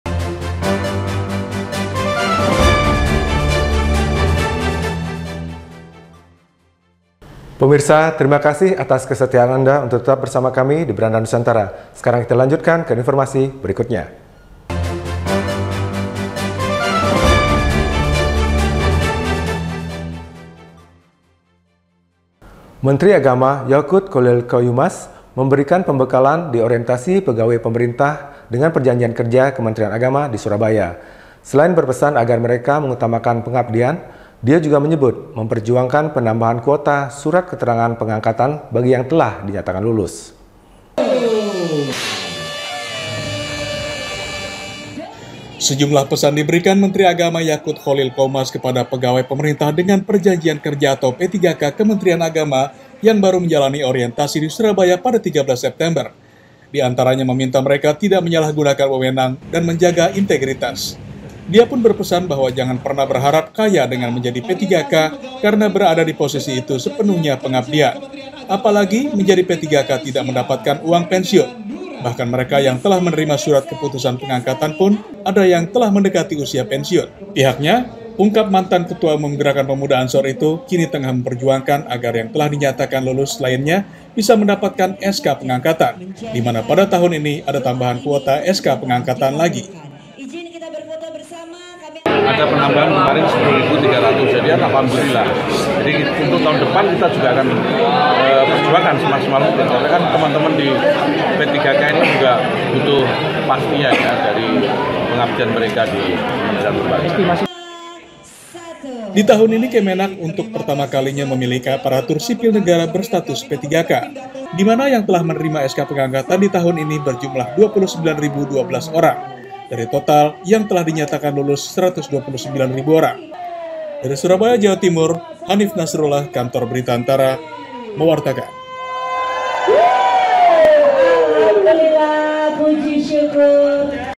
MENAG SEBUT PERJUANGKAN PENAMBAHAN SK PENGANGKATAN PPPK || BERANDA NUSANTARA
Menteri Agama Yaqut Cholil Qoumas memberikan pembekalan di Orientasi Pegawai Pemerintah dengan Perjanjian Kerja (PPPK) Kementerian Agama di Surabaya, Rabu (13/9). Selain berpesan agar mereka mengutamakan pengabdian, dia juga menyebut memperjuangkan penambahan kuota surat keterangan pengangkatan bagi yang telah dinyatakan lulus.